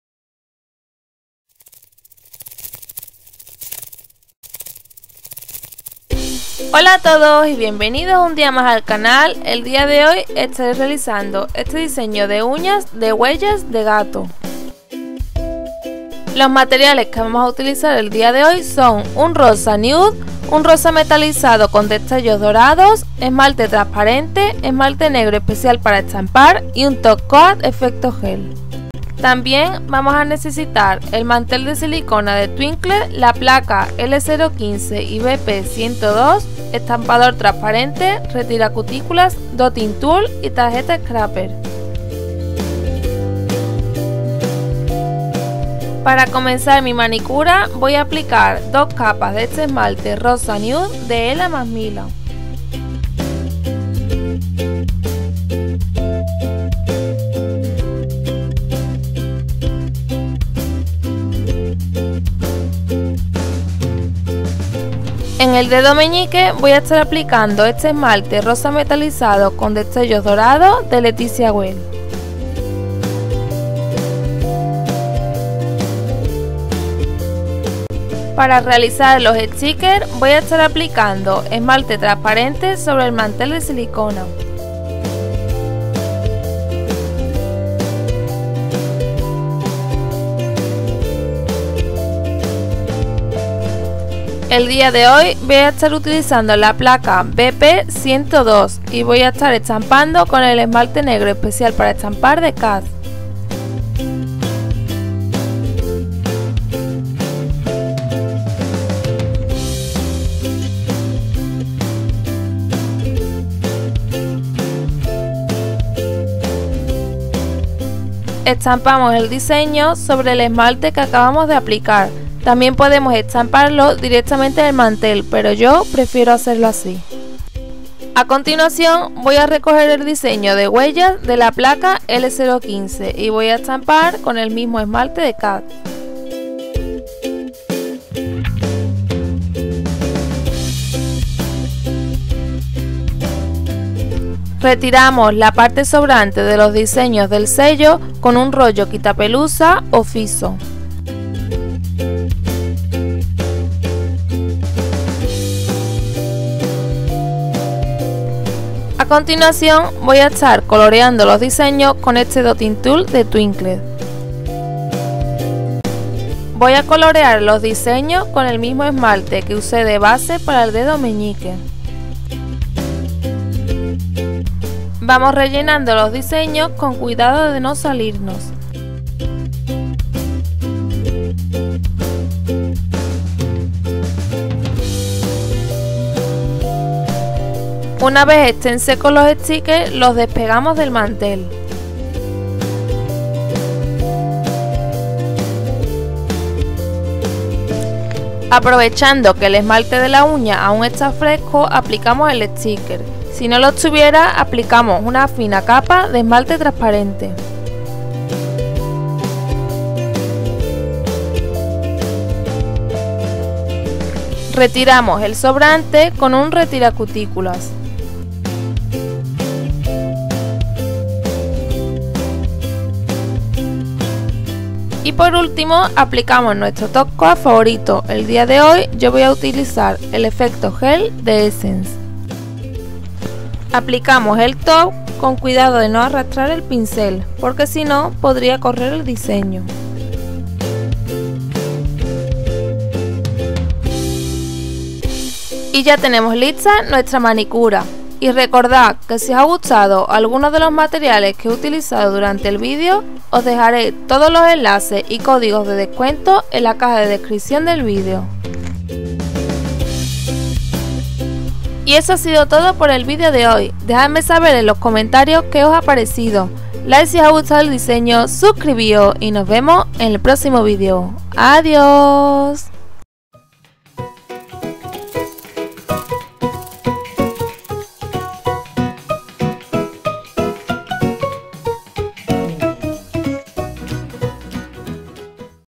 Diseño de Uñas Huellas de Gato Rose Gold
♥ ♥ LÉEME / DESPLIEGAME ♥ ♥

♡ LISTA DE MATERIALES:

- Esmalte transparente Leticia Well XXL nº 300
- Esmalte Top Coat efecto gel de Essence
- Esmalte rose gold Leticia Well XXL nº 463
- Esmalte negro especial para estampar de KADS

- Lápiz de cera para decoración

- Contenedor triángulo